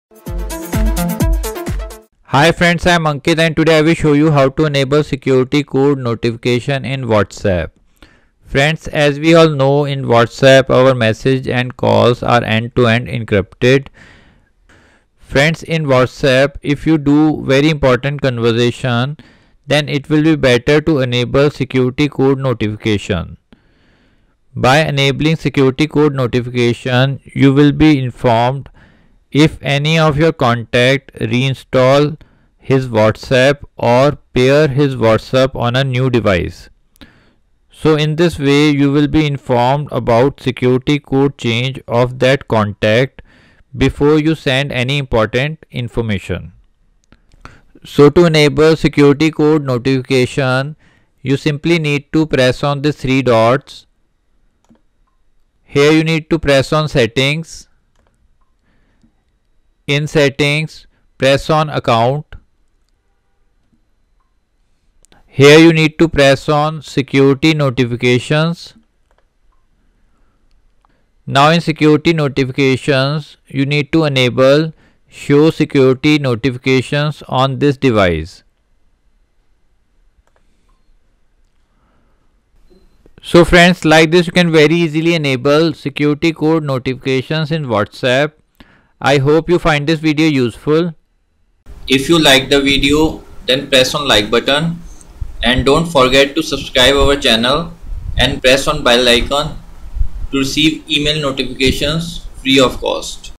How to enable Security Code Notifications in Whatsapp
In this video we have shown How to enable Security Code Notifications in Whatsapp Step by Step